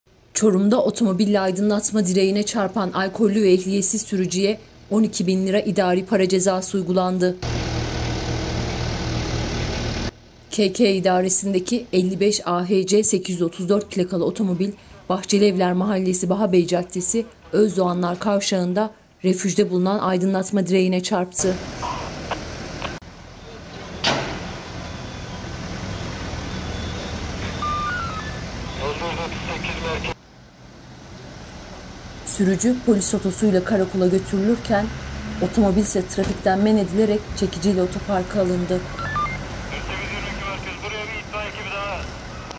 HEM EHLİYETSİZ HEM DE ALKOLLÜ! BİNLERCE LİRA CEZA YEDİ
Çorum'da otomobille aydınlatma direğine çarpan alkollü ve ehliyetsiz sürücüye 12 bin lira idari para cezası uygulandı.
K.K. (22) idaresindeki 55 AHC 834 plakalı otomobil, Bahçelievler Mahallesi Bahabey Caddesi Özdoğanlar Kavşağı'nda refüjde bulunan aydınlatma direğine çarptı.
İhbar üzerine olay yerine polis ekipleri sevk edildi.
Ekiplerce yapılan kontrolde ehliyetinin olmadığı ve alkollü olduğu belirlenen sürücüye toplam 12 bin lira idari para cezası uygulandı.
Sürücü polis otosuyla karakola götürülürken, otomobil ise trafikten men edilerek çekici ile otoparka alındı.